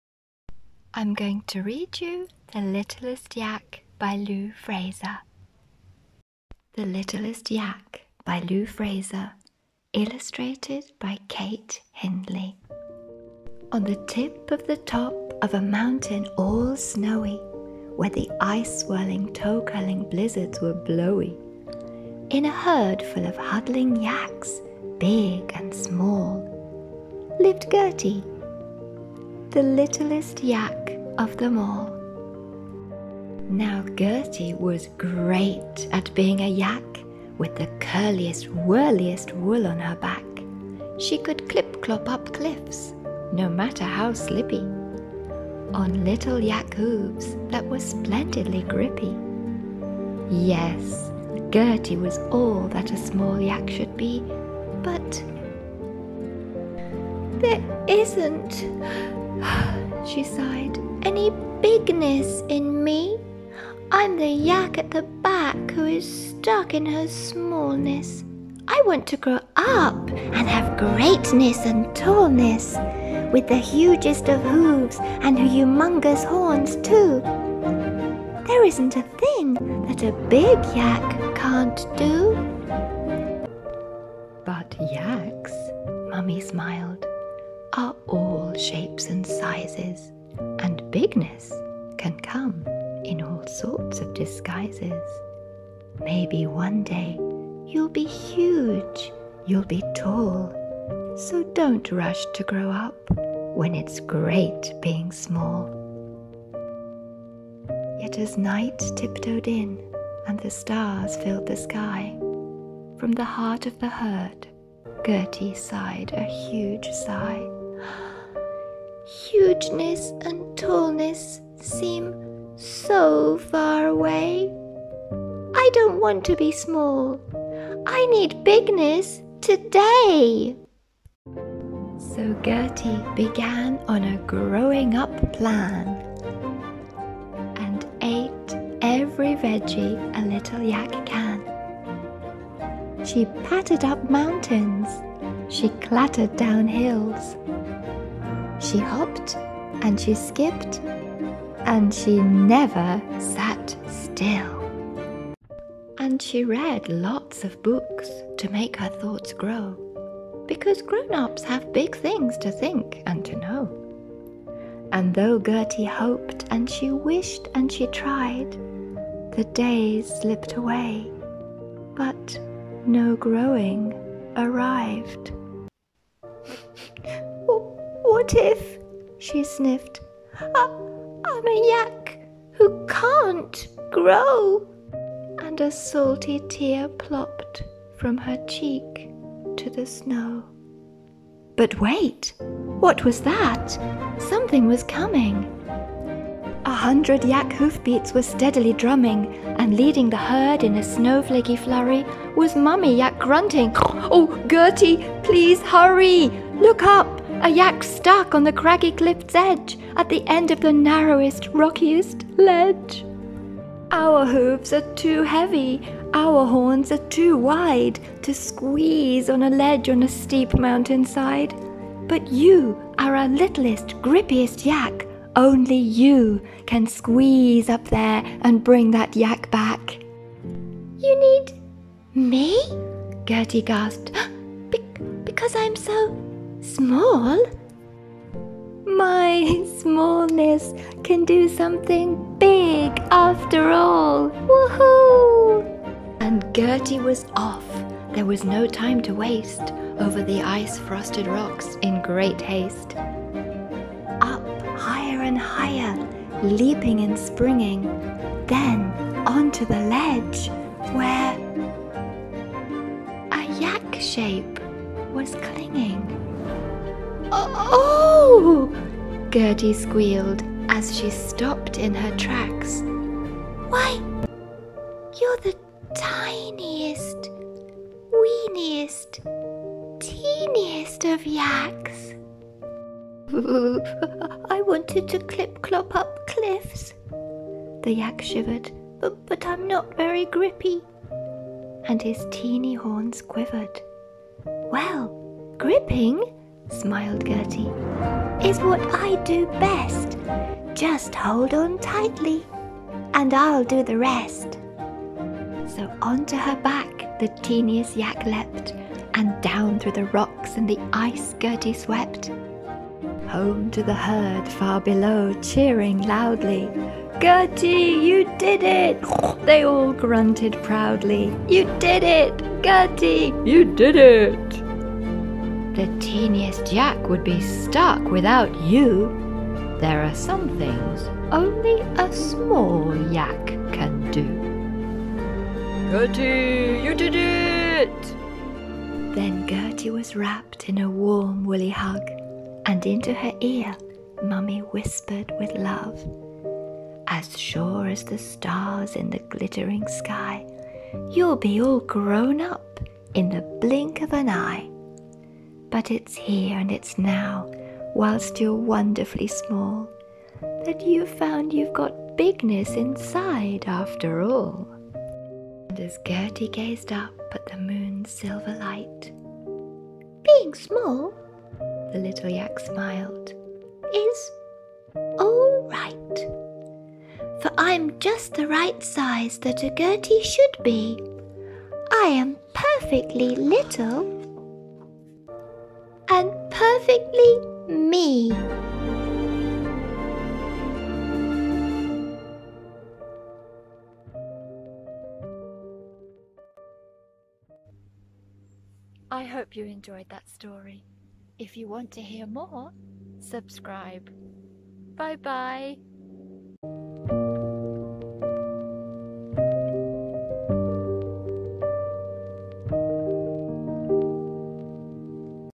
Story time : The Littlest Yak
A heartwarming story about how the littlest yak Gertie is impatient to grow up. She longs to be big. Very soon her smallness is just what is needed. We share her pride, as she discovers her own greatness. Being small is not so bad after all.
Read on location in snow covered mountain woodlands.